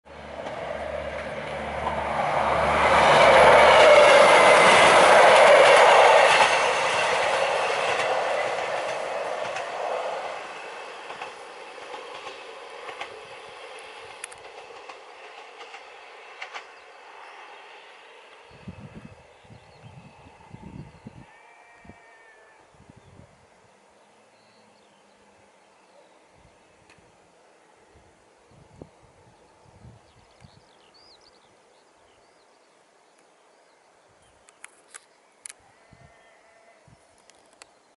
A southbound Transport for Wales train heads towards Tywyn Station Gwynedd Cymru/Wales 26.6.24
A southbound Transport for Wales train heads towards Tywyn Station Gwynedd Cymru/Wales, filmed on 26.6.24 from the estuary bridges road.